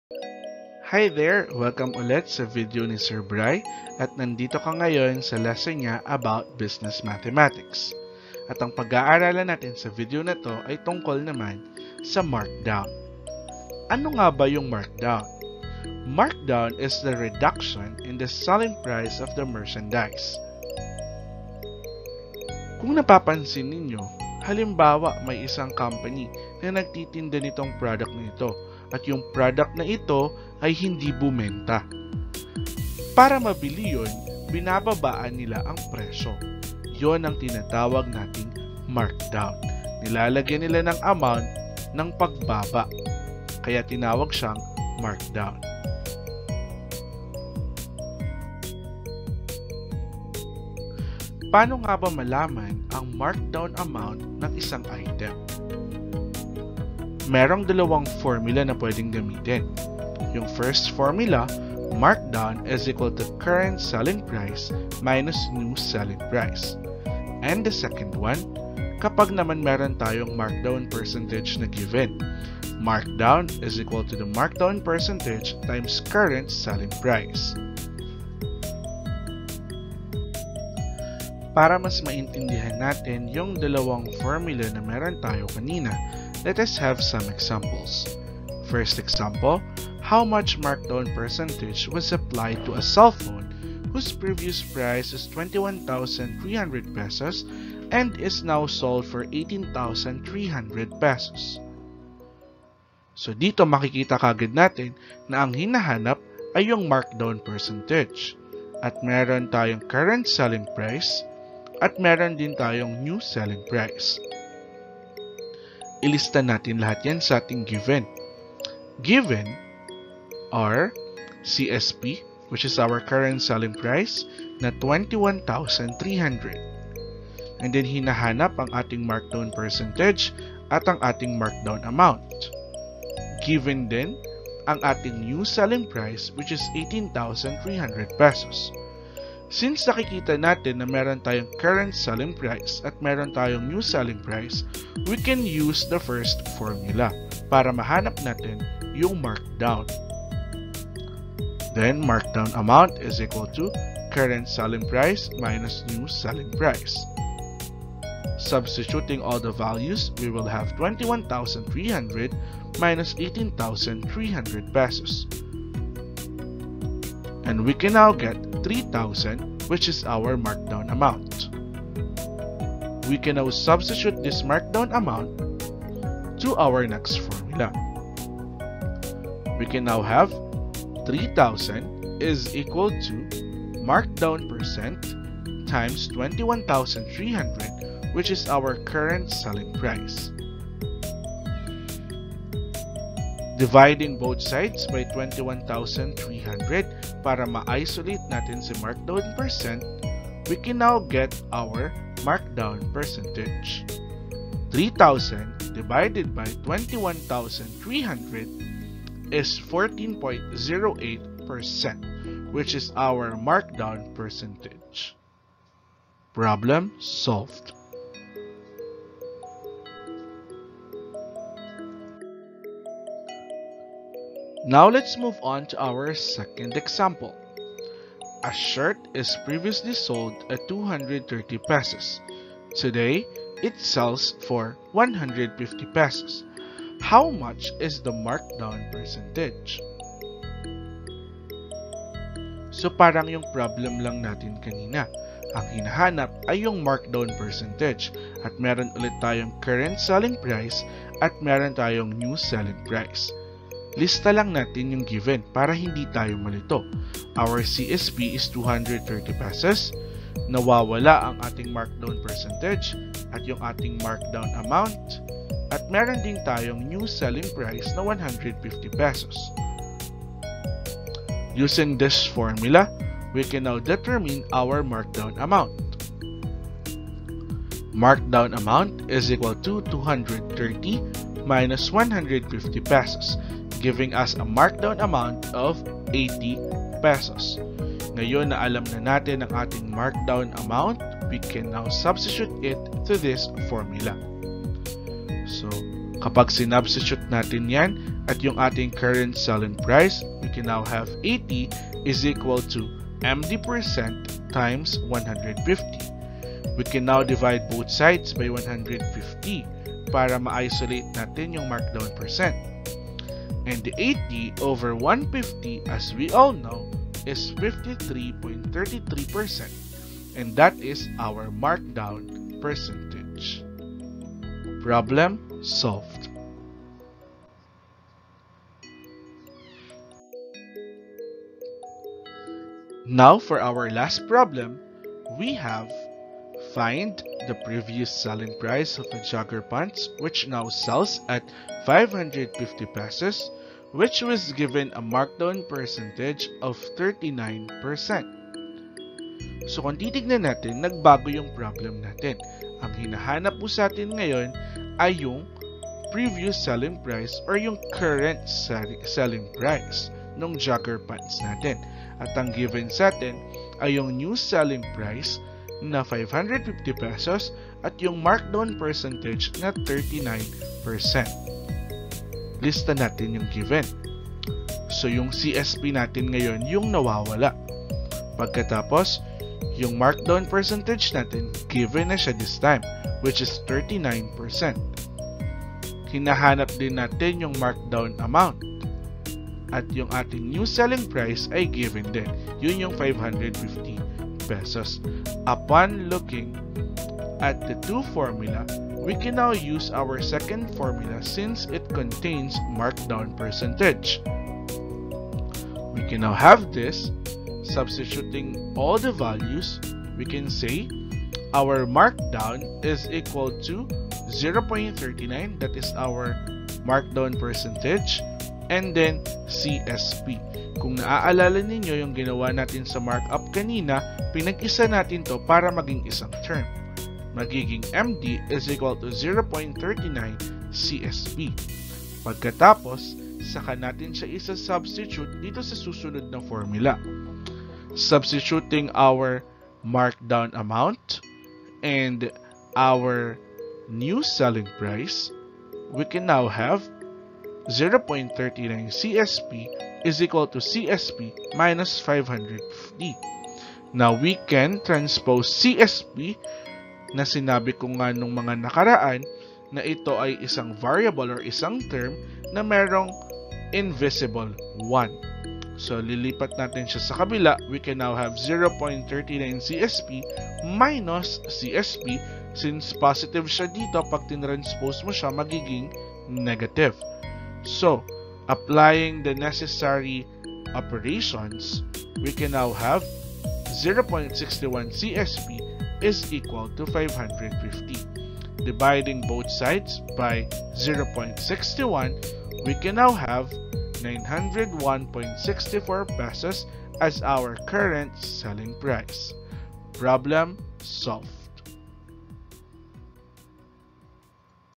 Markdown and Examples (Business Mathematics - Tagalog)
Learn about markdown and solve for it through different examples.